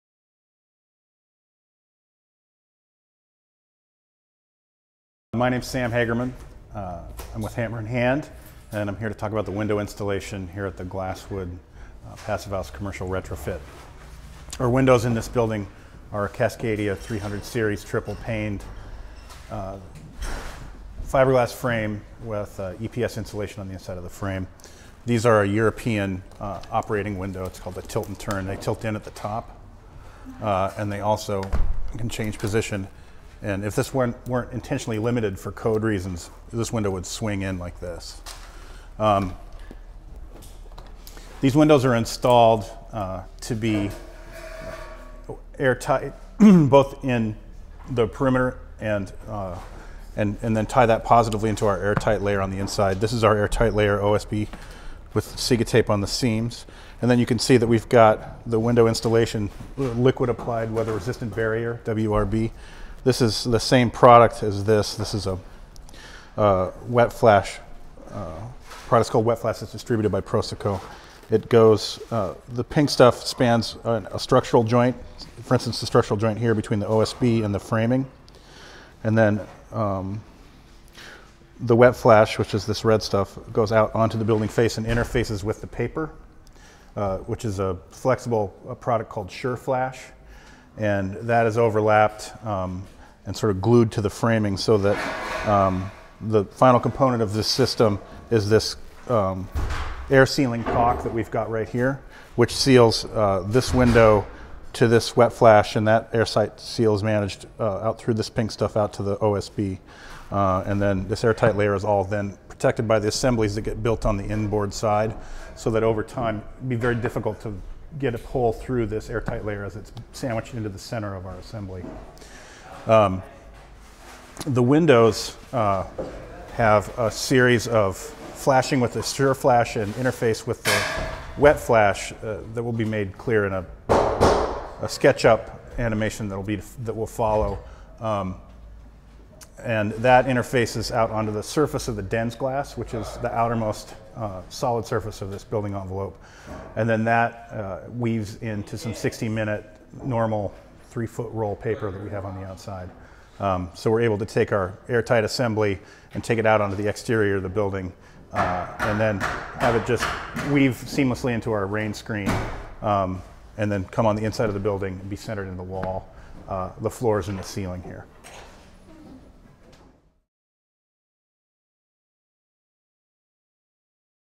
Glasswood Commercial Passive House Retrofit: Window Installation
Sam Hagerman (co-owner of Hammer & Hand, and President of Passive House Alliance US) explains the window installation used on location at the Glasswood commercial Passive House retrofit project.